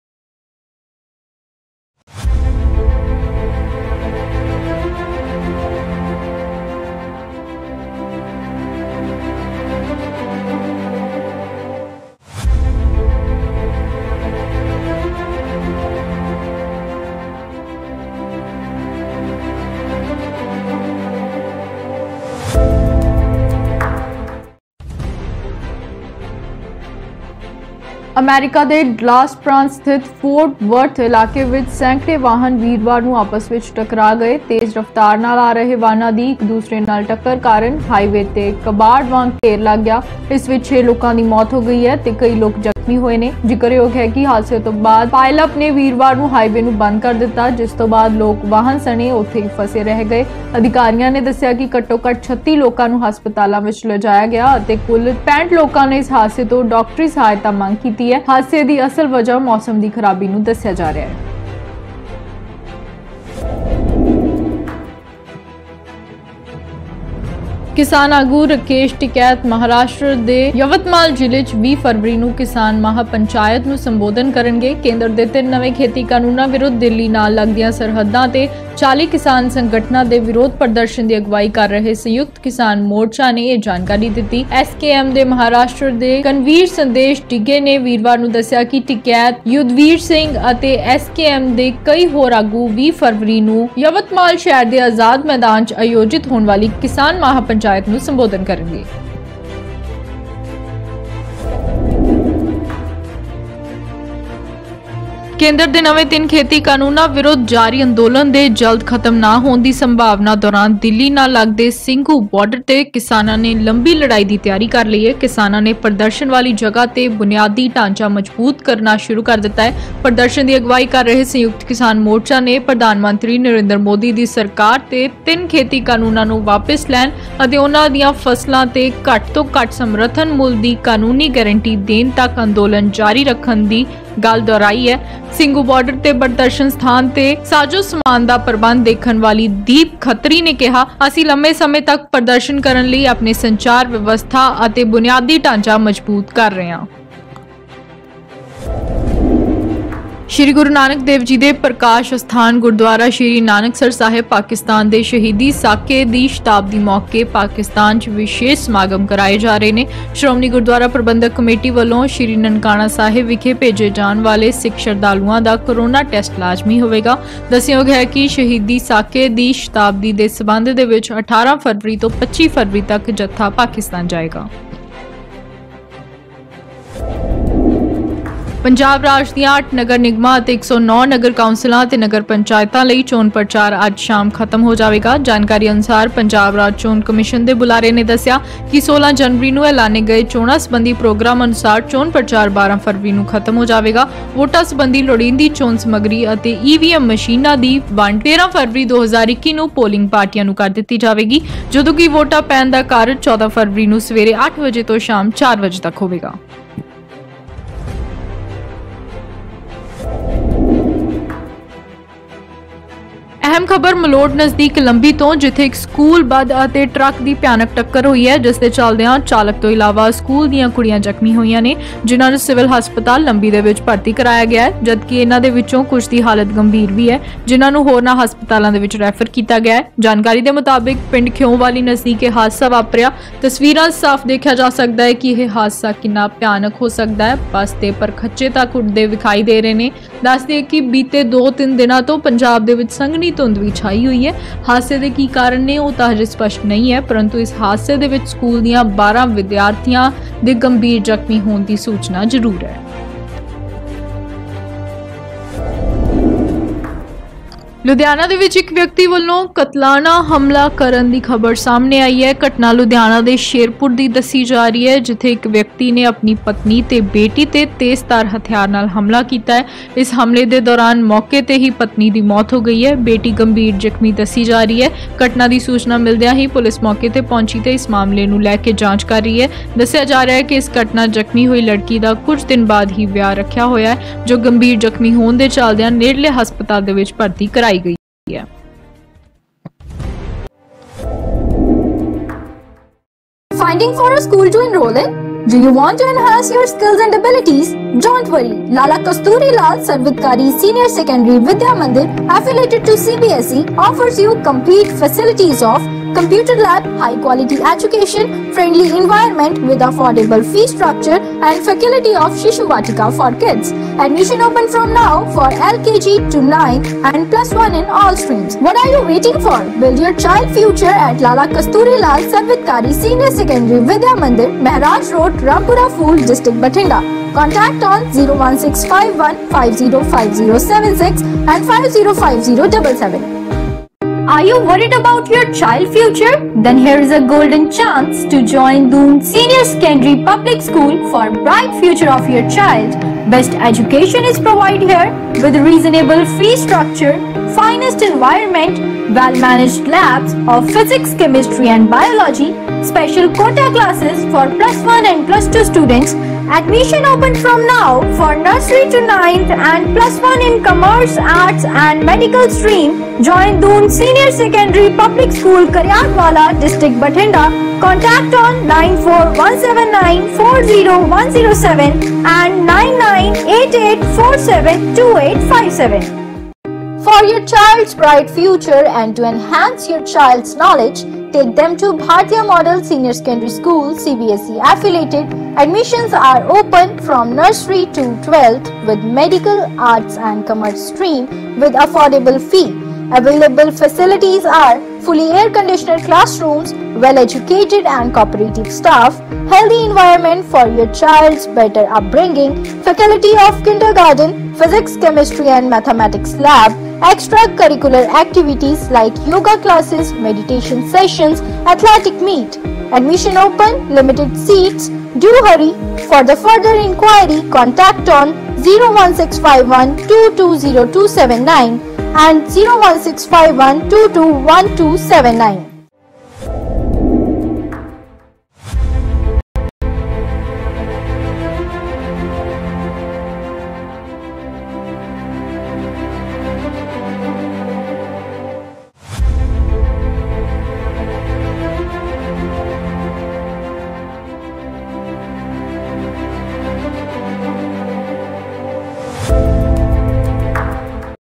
👉👉 ਅਮਰੀਕਾ ’ਚ ਵਾਪਰਿਆ ਵੱਡਾ ਹਾਦਸਾ, || Punjabi News Bulletin || Live
👉👉 ਅਮਰੀਕਾ ’ਚ ਵਾਪਰਿਆ ਵੱਡਾ ਹਾਦਸਾ, 100 ਤੋਂ ਵੱਧ ਵਾਹਨਾਂ ਦੀ ਟੱਕਰ, ਦਰਜਨਾਂ ਲੋਕਾਂ ਦੀ ਗਈ ਜਾਨ
👉👉 ਰਾਕੇਸ਼ ਟਿਕੈਤ ਮਹਾਰਾਸ਼ਟਰ 'ਚ 20 ਫਰਵਰੀ ਨੂੰ 'ਕਿਸਾਨ ਮਹਾਪੰਚਾਇਤ' ਨੂੰ ਕਰਨਗੇ ਸੰਬੋਧਨ
👉👉 ‘ਲੰਬੀ ਲੜਾਈ’ ਲਈ ਤਿਆਰ ਕਿਸਾਨ, ਪ੍ਰਦਰਸ਼ਨ ਵਾਲੀ ਜਗ੍ਹਾ ’ਤੇ ਬੁਨਿਆਦੀ ਢਾਂਚਾ ਕਰ ਰਹੇ ਮਜ਼ਬੂਤ
👉👉 ਅਹਿਮ ਖ਼ਬਰ : ਸ੍ਰੀ ਨਨਕਾਣਾ ਸਾਹਿਬ ਜਾਣ ਵਾਲੇ ਸ਼ਰਧਾਲੂਆਂ ਲਈ ਕੋਰੋਨਾ ਟੈਸਟ ਕਰਵਾਉਣਾ ਜ਼ਰੂਰੀ
👉👉 ਪੰਜਾਬ 'ਚ ਅੱਜ ਸ਼ਾਮ ਖ਼ਤਮ ਹੋ ਜਾਵੇਗਾ 'ਚੋਣ ਪ੍ਰਚਾਰ', 14 ਫਰਵਰੀ ਨੂੰ ਪੈਣਗੀਆਂ ਵੋਟਾਂ
👉👉 ਸਕੂਲ ਬੱਸ ਤੇ ਟਰੱਕ ਦੀ ਭਿਆਨਿਕ ਟੱਕਰ, 12 ਵਿਿਦਆਰਥਣਾਂ ਗੰਭੀਰ ਜਖਮੀਂ
👉👉 ਲੁਧਿਆਣਾ ‘ਚ ਇੱਕ ਸ਼ਖਸ ਨੇ ਪਤਨੀ ਤੇ ਬੇਟੀ ਤੇ ਕੀਤਾ ਹਮਲਾ, 1 ਦੀ ਮੌਤ 1 ਗੰਭੀਰ ਜਖਮੀਂ